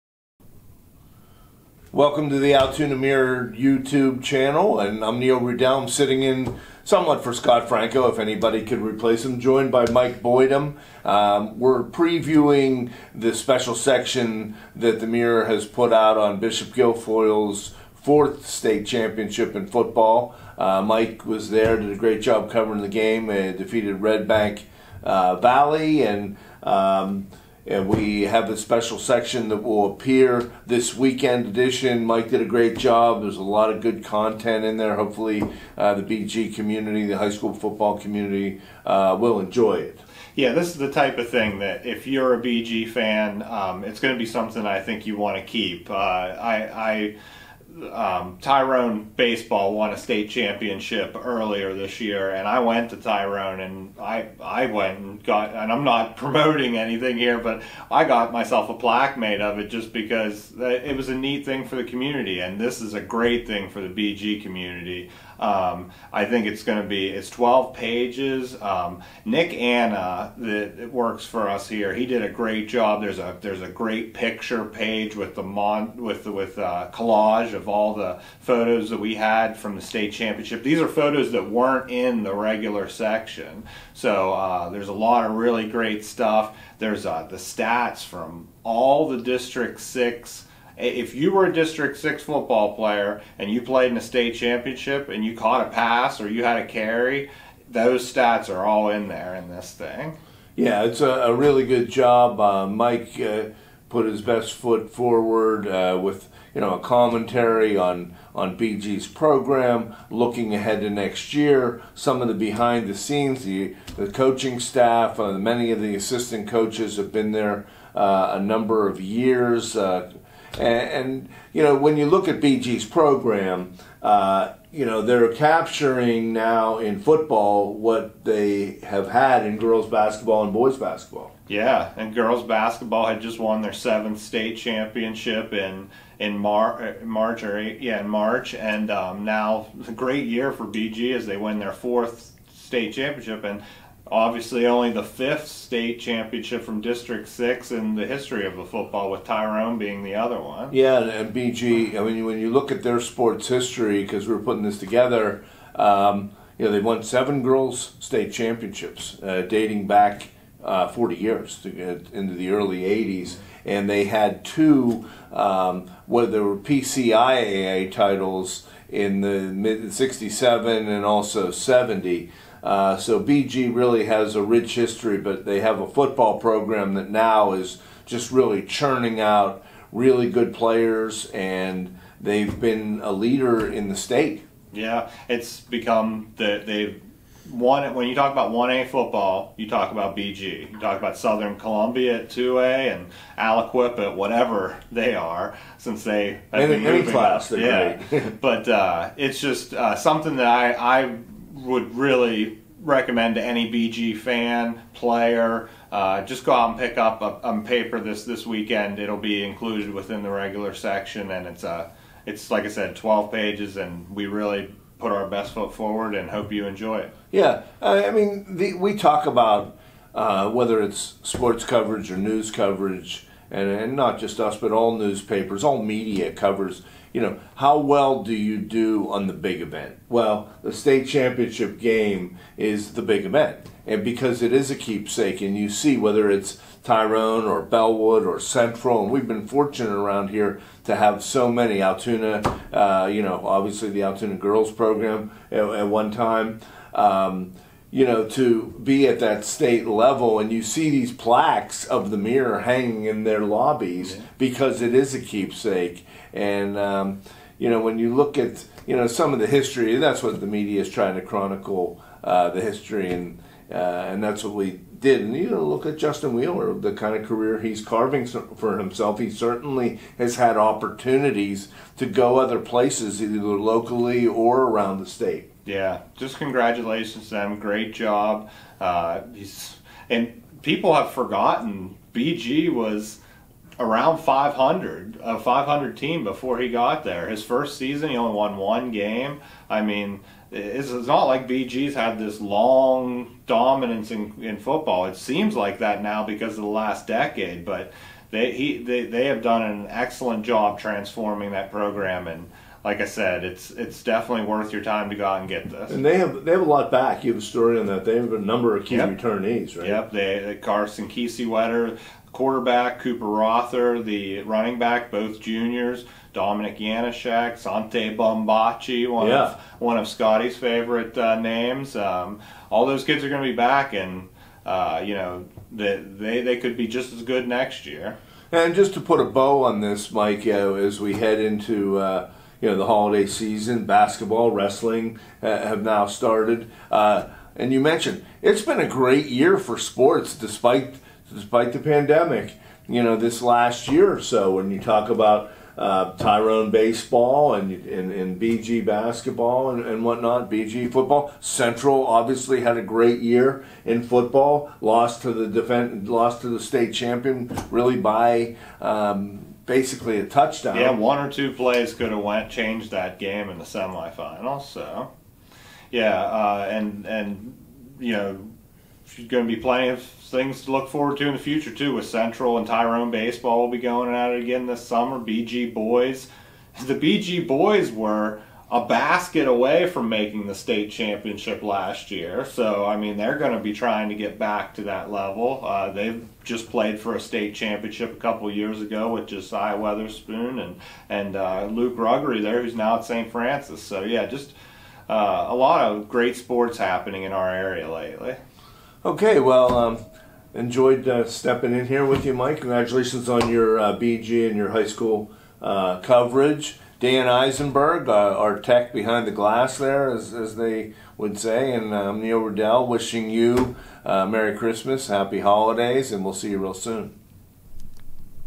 Mike and Neil preview the Bishop Guilfoyle championship special section
HS sports reporter Mike Boytim and managing editor Neil Rudel sit down to preview the Altoona Mirror's special tribute section for the 2021 PIAA 1A Champion Bishop Guilfoyle Marauders.

12 pages of stories and photos recapping the run to the fourth football championship in program history, coming in this weekend's edition of the Mirror. Make sure to get you copy!